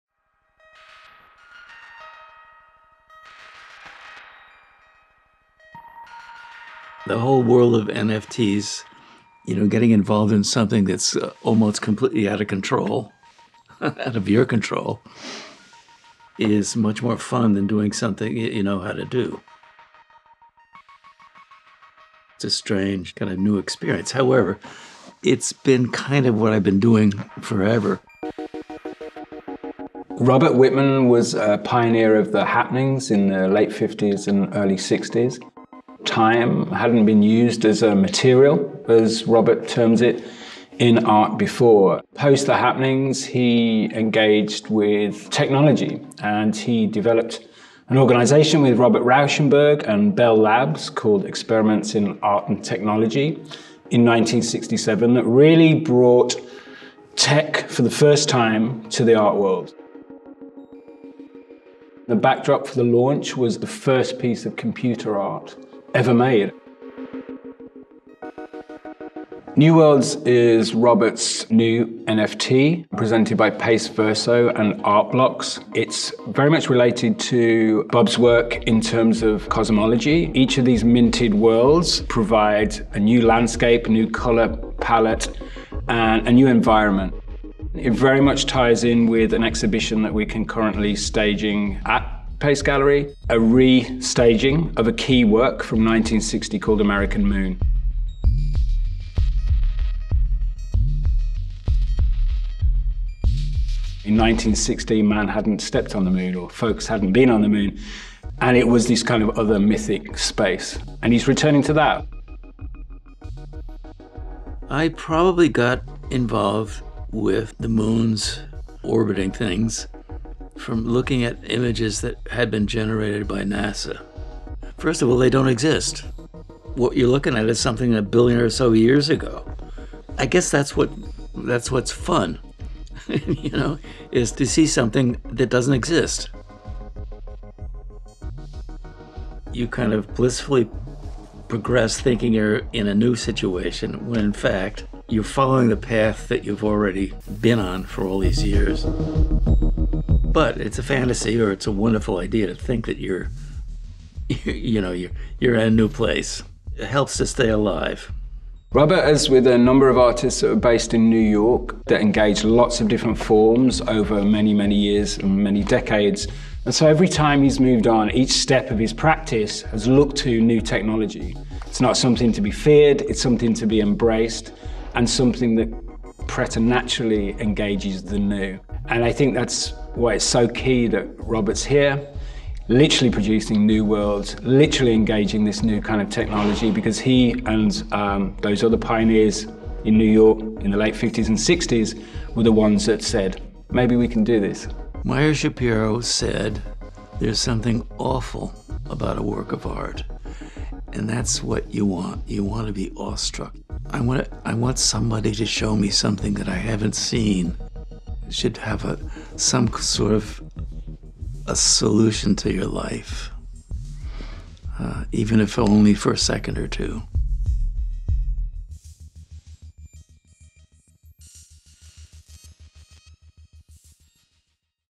New Worlds: Robert Whitman’s First NFTs
In our new film, artist Robert Whitman, known for his collaborative and deeply experimental work across performance and installation, discusses his first-ever web3 project. Titled New Worlds, Whitman's series of 500 generative, interactive, and unique NFTs centers on imagined celestial bodies in space. The release of New Worlds, realized as part of a multifaceted partnership between Pace Verso, the gallery’s web3 hub, and the leading generative art platform Art Blocks, coincides with the artist's new exhibition and an accompanying Pace Live performance series at our New York gallery. Featuring commentary from Pace Live Curatorial Director Mark Beasley, the following film sheds light on Whitman’s contributions to the histories of performance and technologically engaged art making.